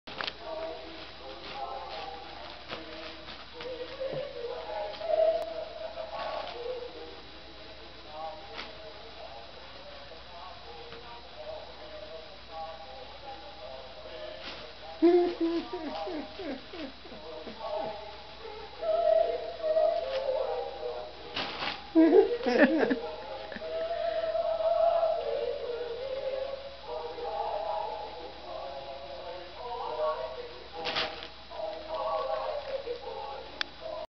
Gwinny and her Christmas ritual
Gwinny LOVES Christmas wrapping paper